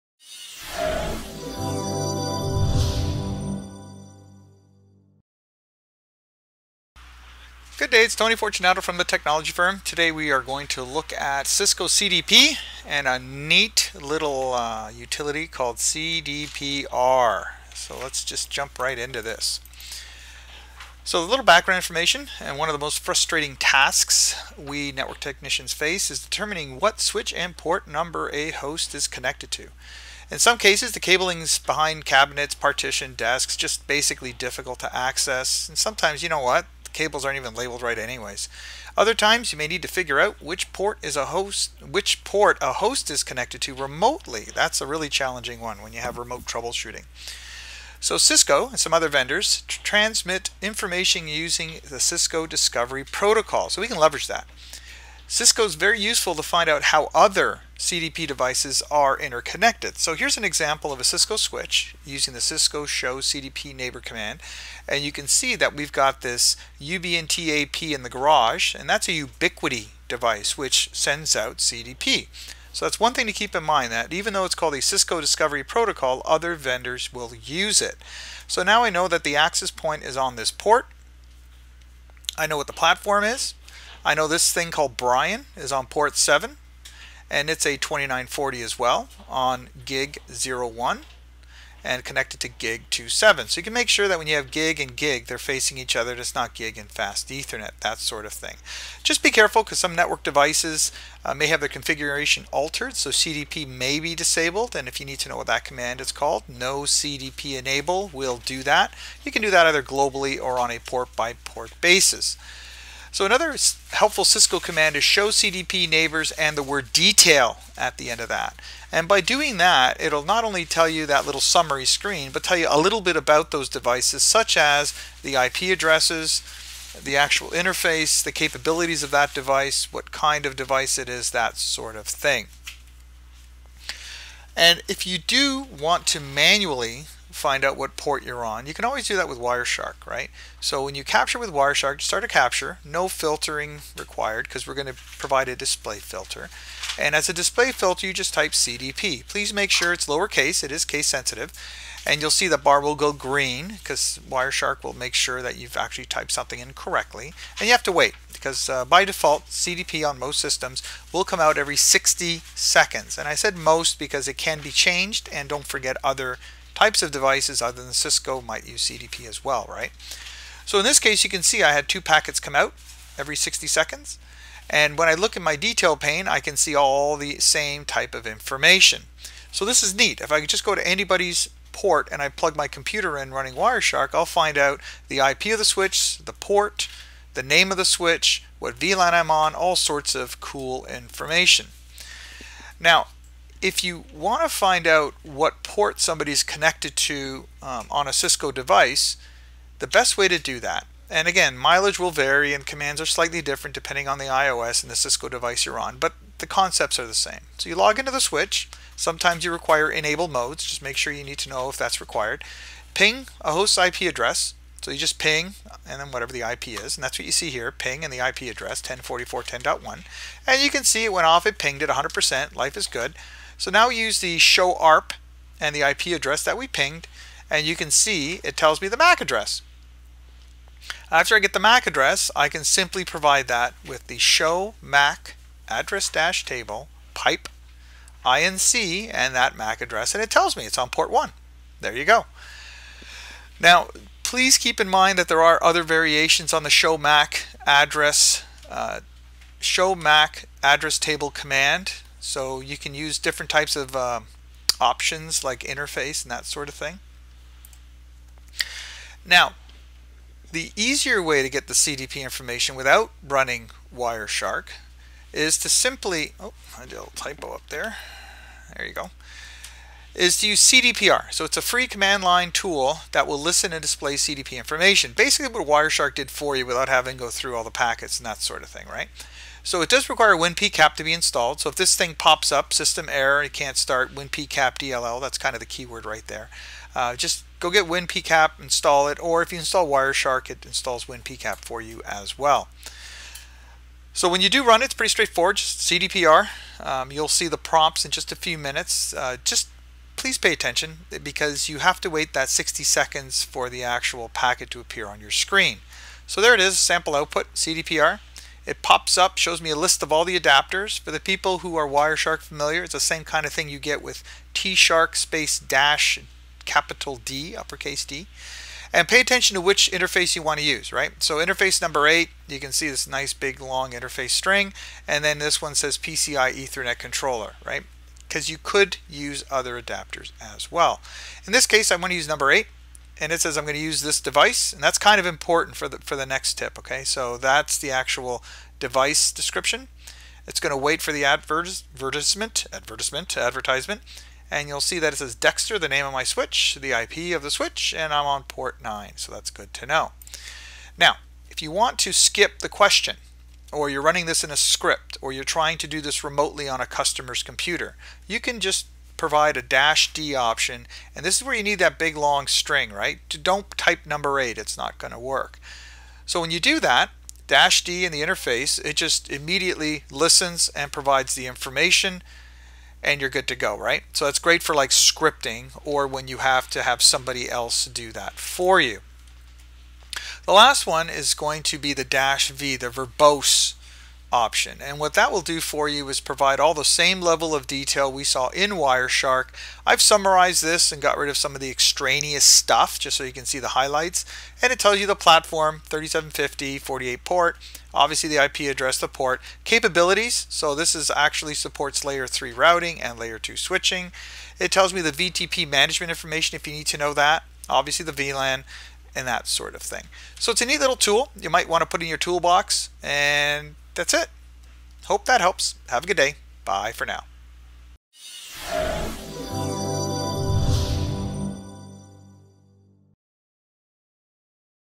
How Can Cisco Discovery Protocol Help Me
One of the common challenges network analysts face is determining what port a host is connected to.
It doesn’t matter if it’s the receptionist’s desktop or a firewall port, sooner or later you will encounter a situation where you need to verify, document or determine what port a device is connected to.
If you’re lucky you can look at the cable or jack identifier, reference you network documentation and you are good to go. But what do you do when you can’t easily access the cables, they aren’t labeled or you believe they aren’t labeled correctly? To add to the mix, what do you do for remote troubleshooting?

the rest of the article will be posted on Network Computing